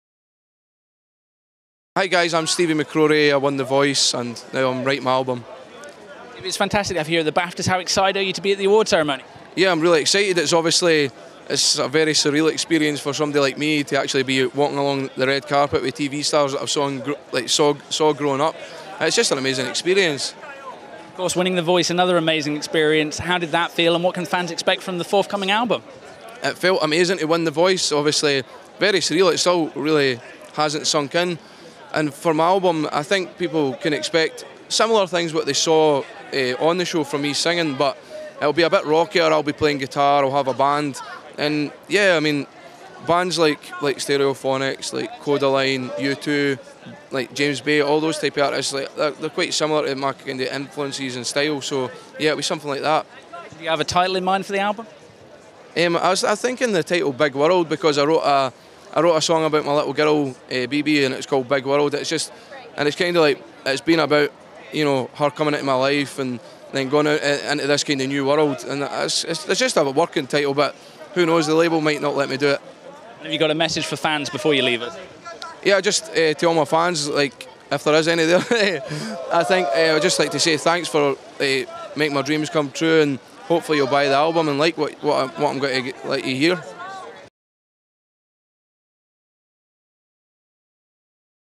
Steve McCrorie Interview - The Voice & New Album
The Voice 2015 winner Steve McCrorie talks to Red Carpet News at the BAFTA Television Awards ceremony in London.